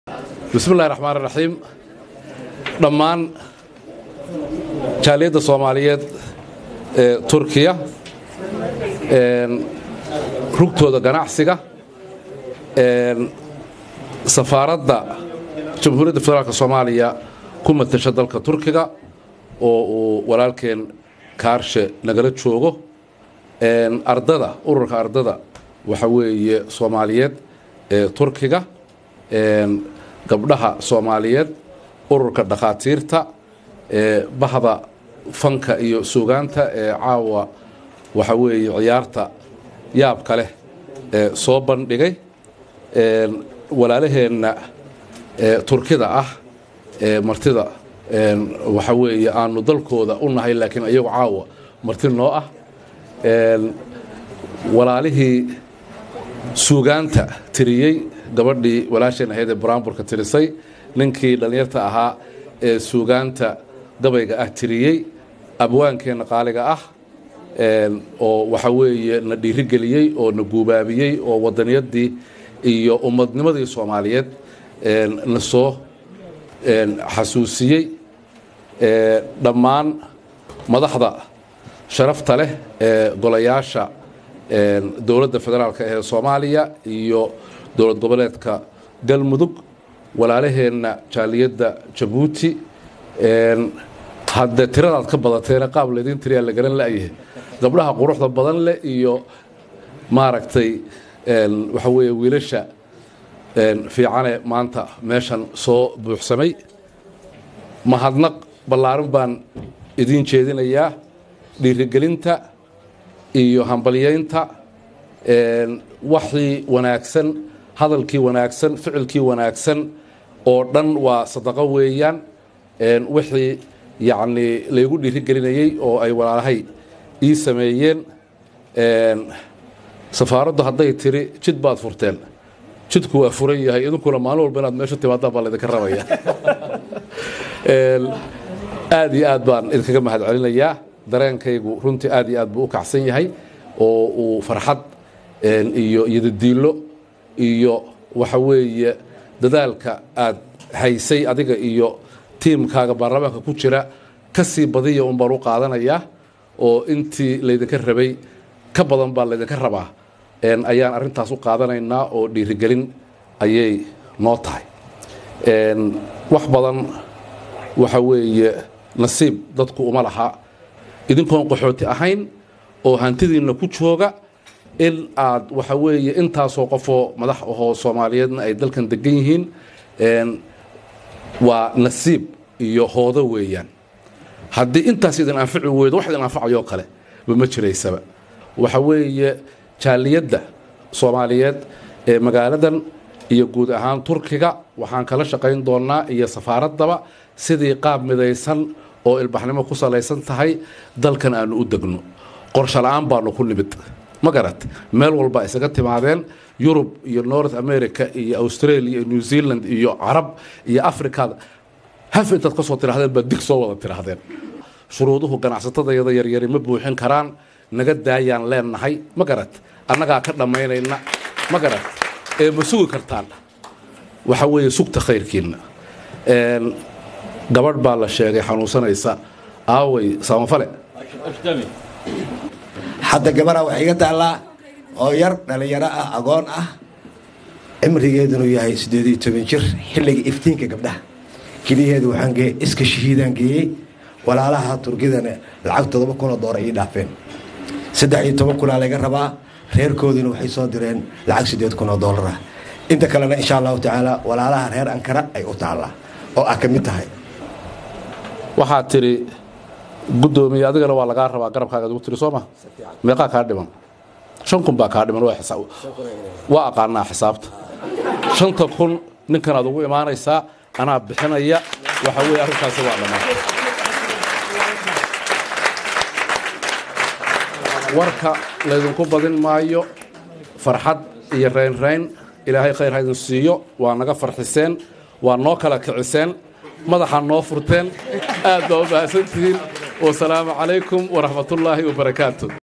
Gudomiye Kuxigeenka labad Ee Dowlada federal Somalia Mahad Cabdalla Cawad Soo dhaweyntii Turkey 🇹🇷
copyright Ayman Channel